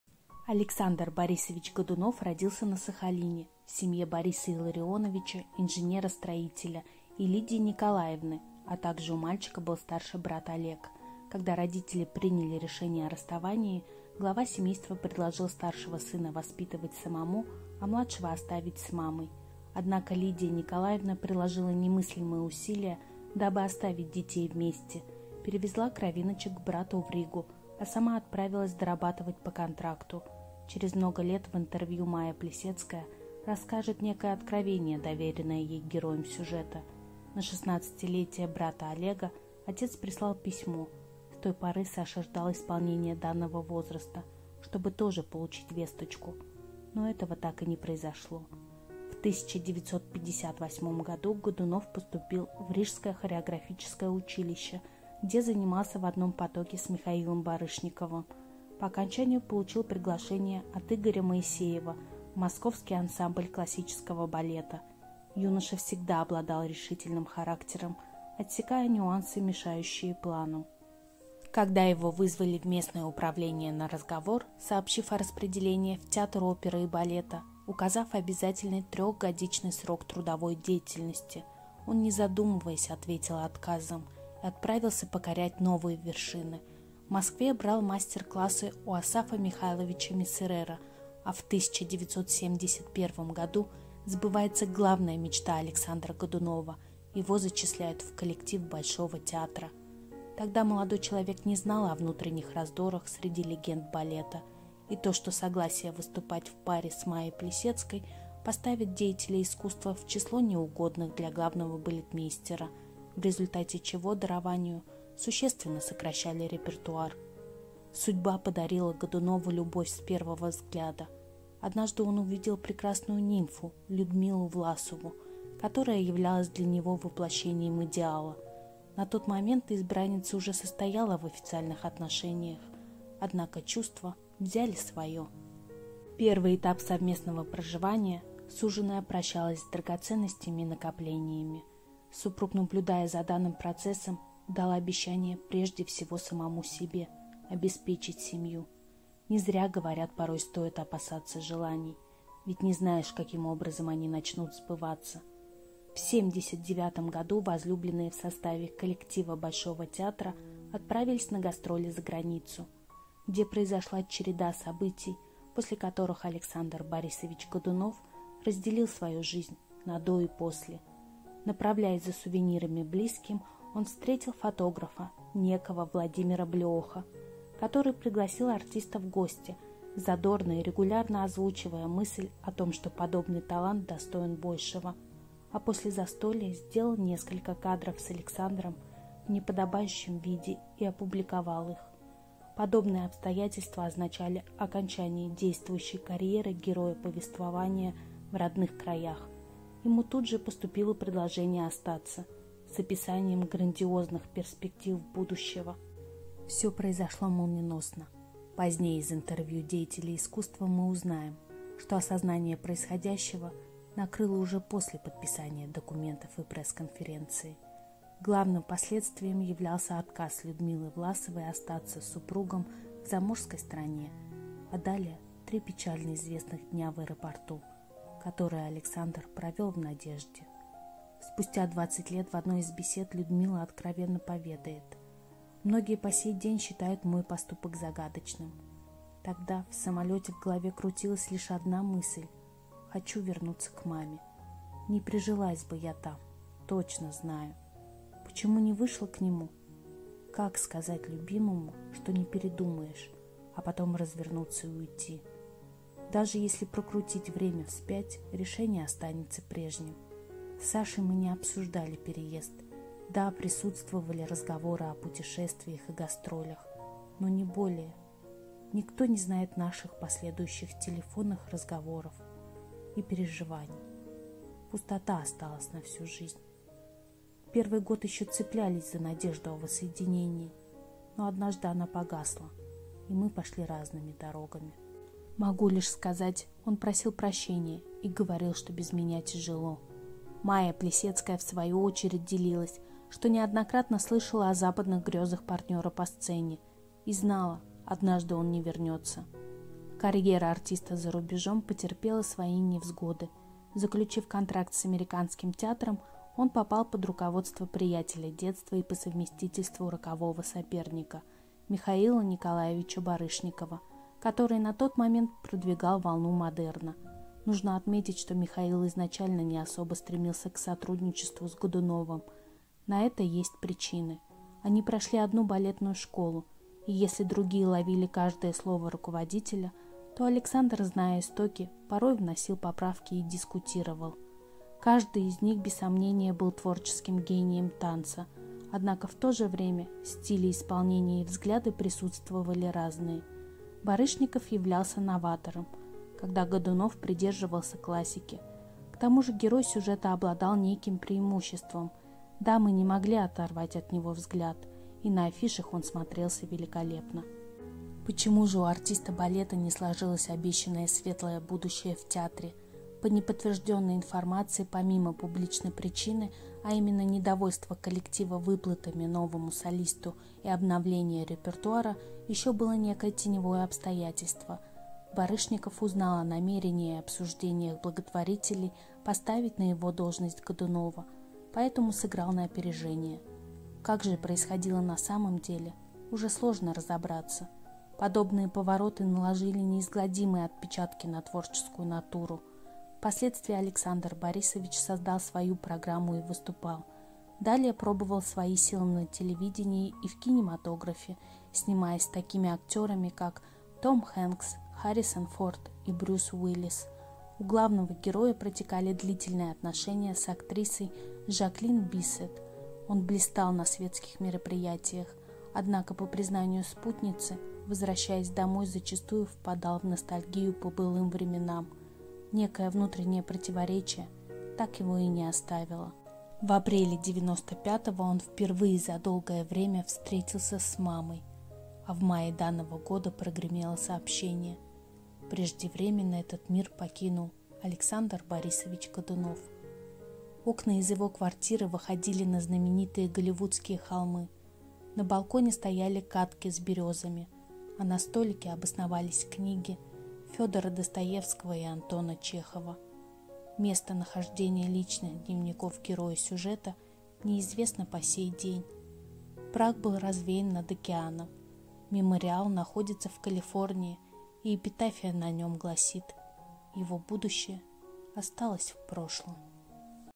Александр Годунов "Горделивый красавец полыхал на ветру неповторимого пируэта."
История жизни артиста балета.